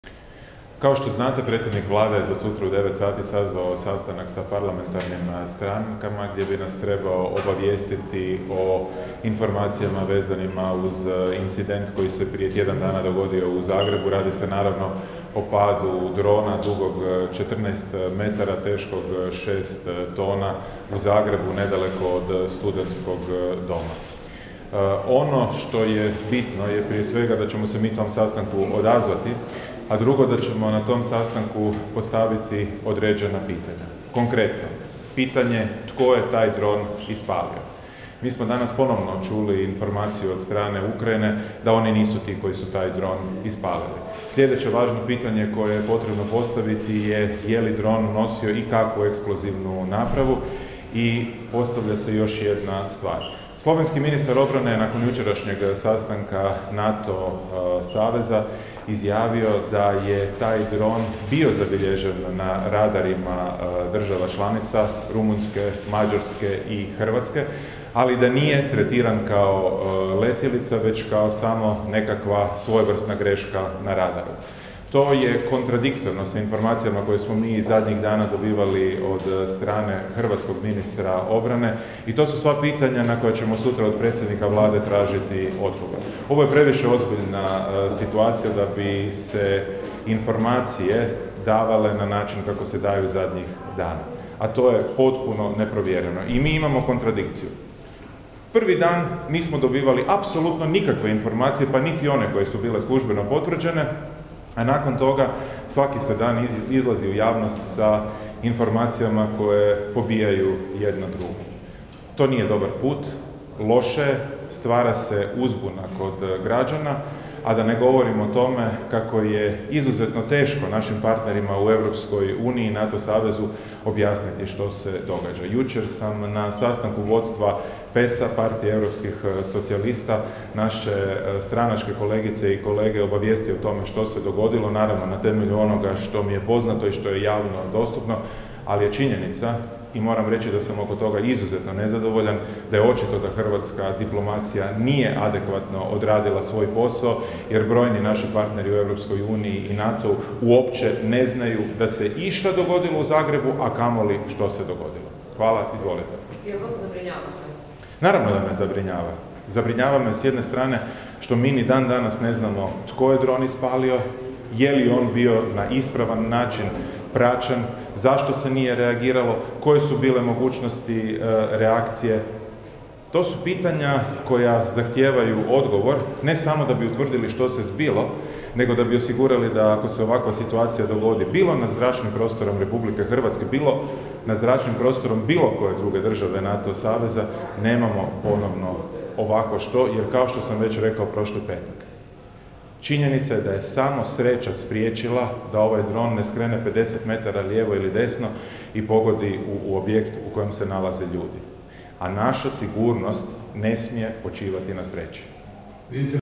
Grbin: Tražit ćemo od Plenkovića odgovor tko je ispalio dron i da li je nosio eksplozivnu napravu
Na konferenciji za medije održanoj u Saboru zastupnik Peđa Grbin komentirao je sutrašnji sastanak oporbe sa premijerom.